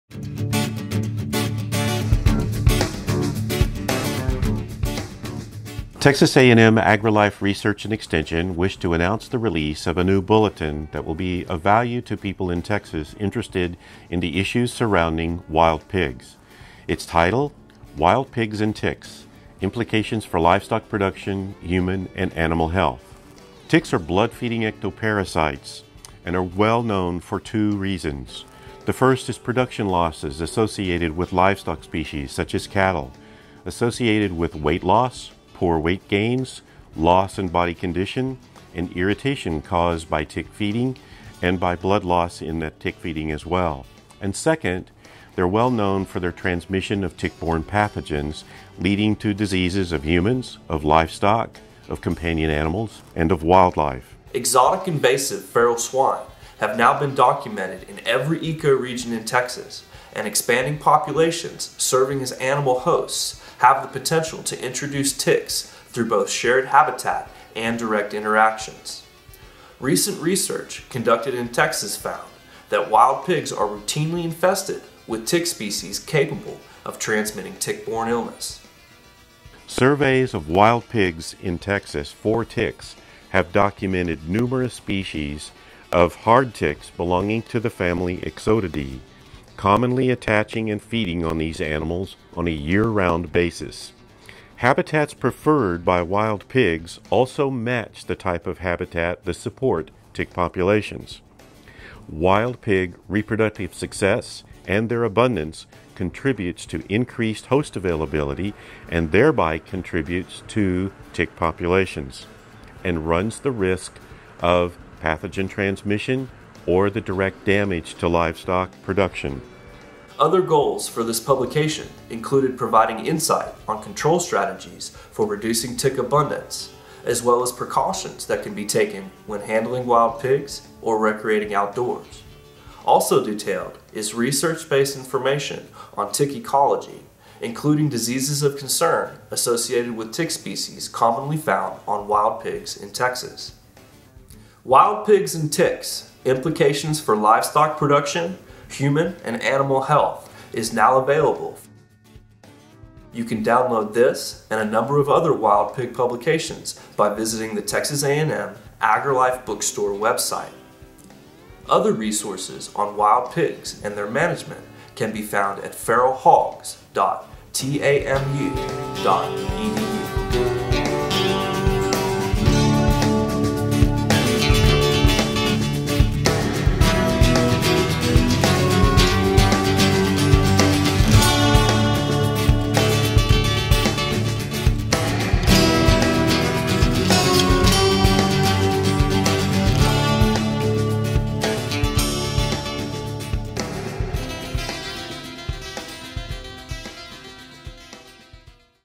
Wild Pigs and Ticks: Implications for Livestock Production, Human and Animal Health
This video highlights our latest publication on the interactions between wild pigs and ticks.